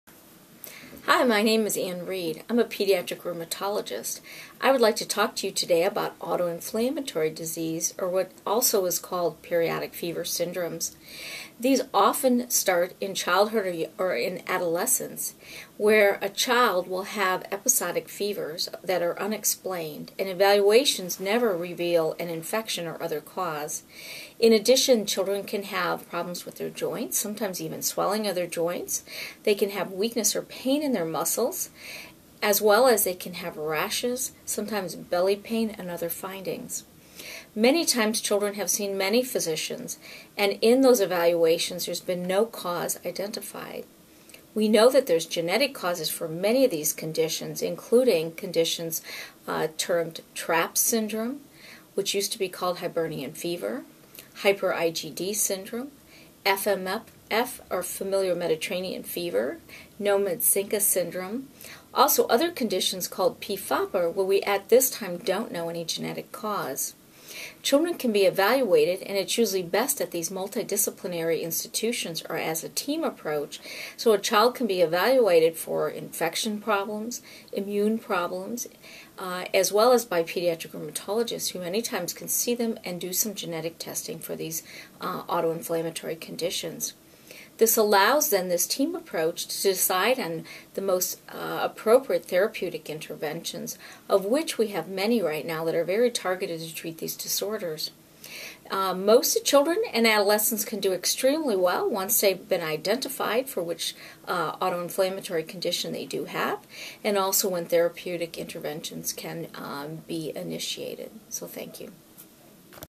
Autoinflammatory Disorders
Pediatric rheumatologist Ann Reed, M.D., of Mayo Clinic Children's Center in Rochester, Minnesota, speaks about the symptoms and treatment options for pediatric autoinflammatory disorders which can also be referred to as periodic fevers.